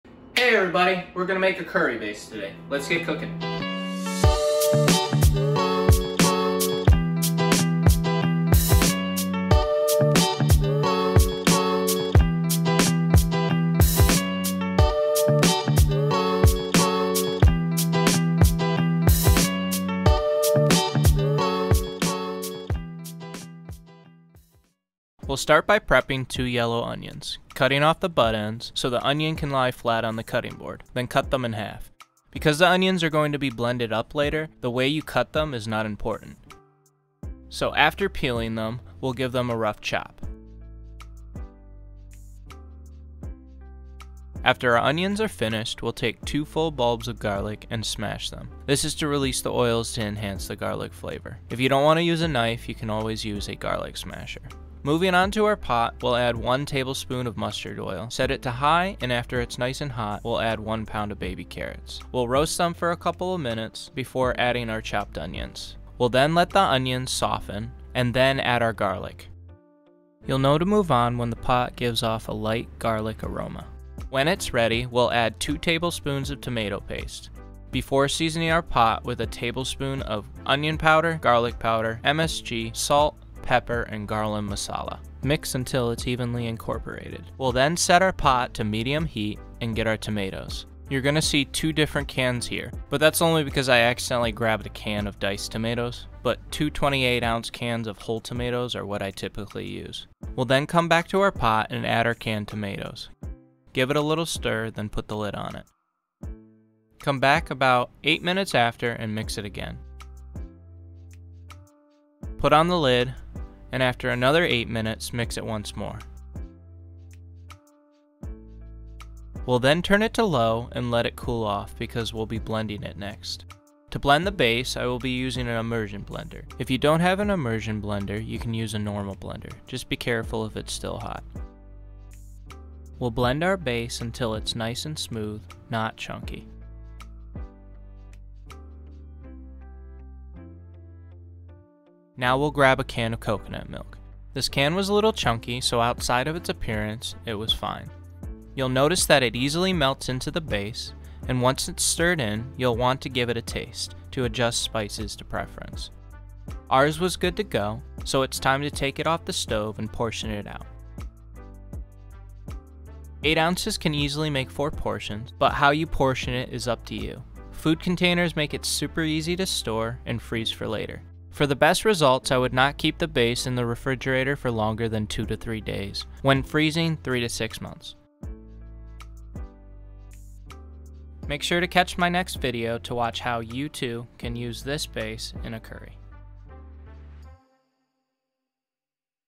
Tomato Curry Base
Creating a curry base can be very convenient--especially when portioning it for later. This recipe can easily be added to your weekly meal prep!